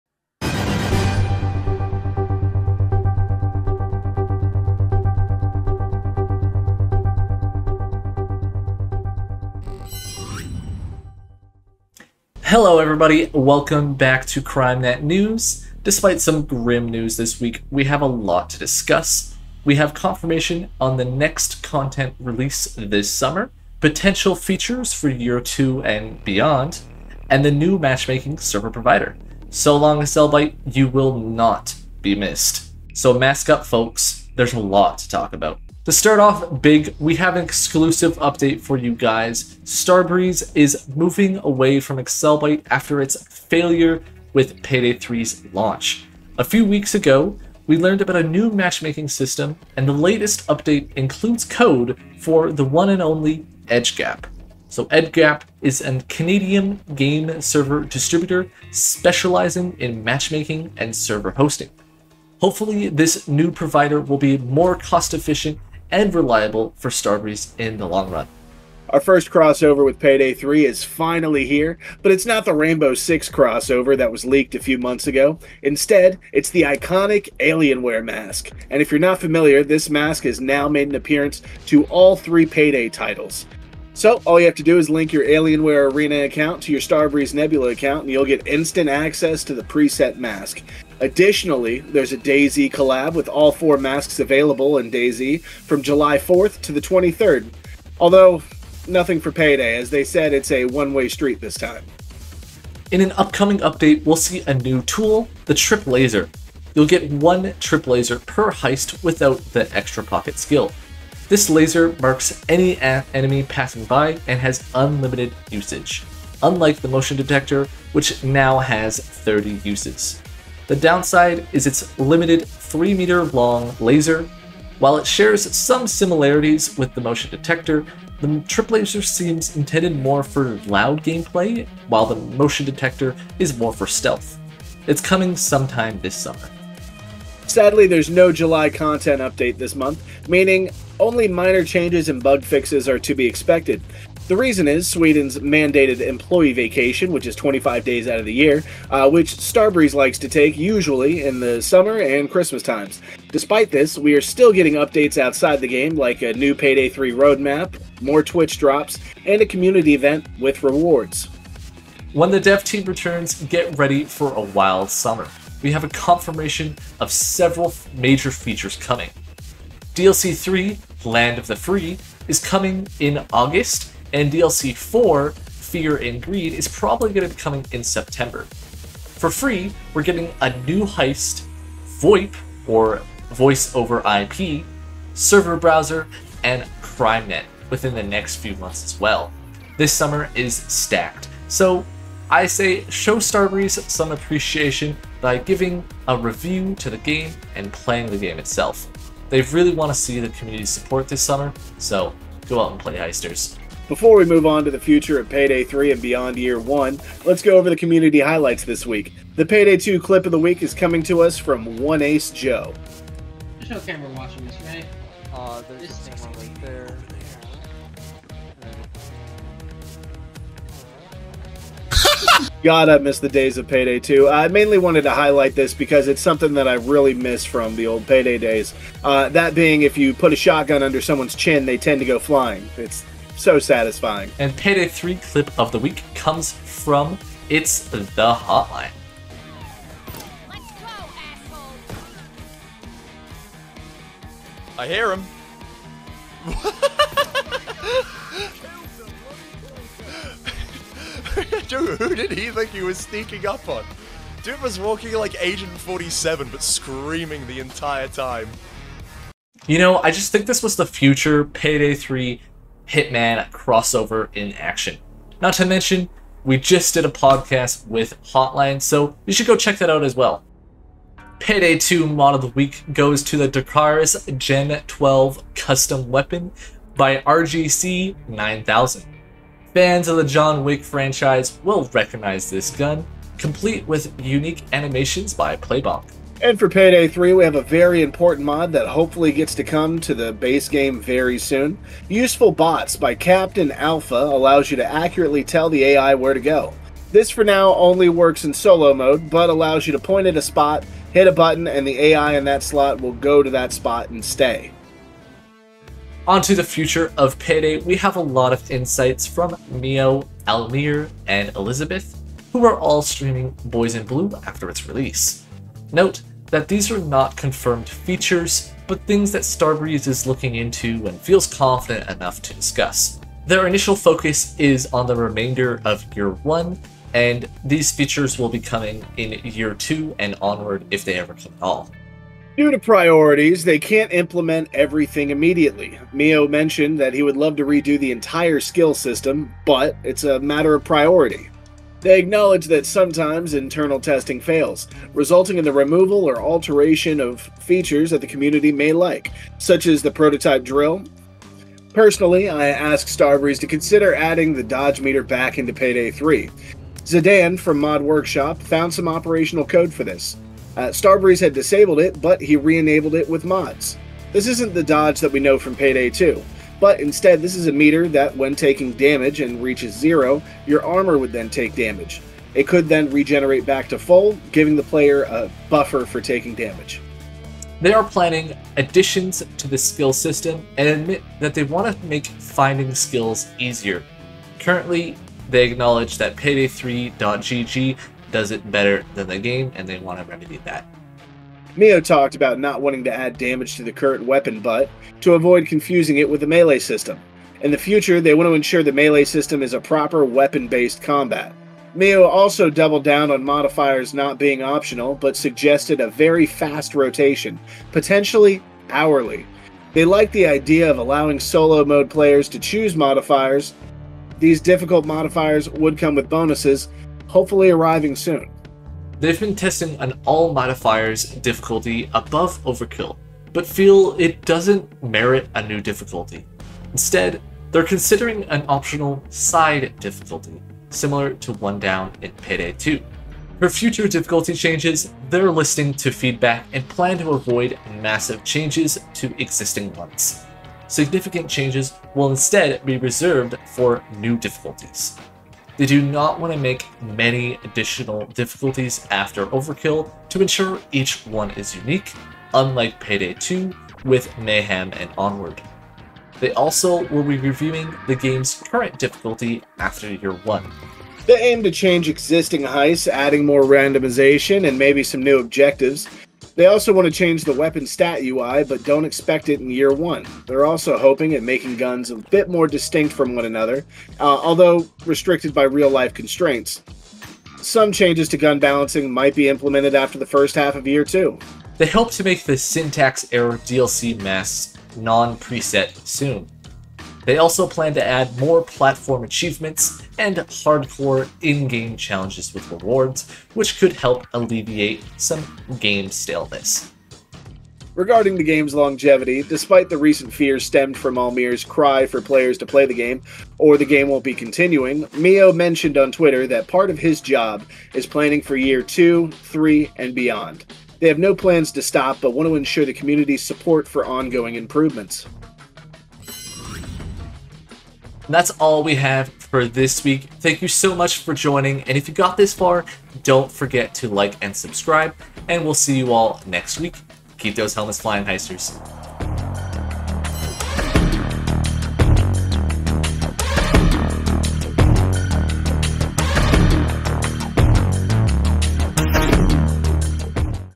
Payday 3 got a new Server Provider | Crime.Net News
Welcome to Crime.Net News. We'll be going over weekly (or hopefully close to that) news going on in payday and starbreeze. Feedback for news segments and bits would be greatly appreciated to hear about in the comments below.

0:00 - 0:35 Intro
0:36 - 1:07 New Server Provider
1:08 - 1:41 Crossovers
1:42 - 2:16 Trip laser
2:17 - 3:24 Summer update schedule
2:56 - 4:30 Clip of the week
4:31 - 5:05 Mod of the week
5:06 - 6:17 Starbreeze wishes they could do more
6:18 - 6:30 Skill system rework
6:31 - 6:41 Melee Weapon system
6:42 - 7:40 Modifiers/Difficulty
7:41 - 7:46 Changes to existing heists
7:47 - 8:01 Weapon changes
8:02 - 8:17 Misc changes
8:18 - 8:36 Mio's comment on the game ending March 2025
8:37 - 9:00 Outro

Week of June 30th, 2024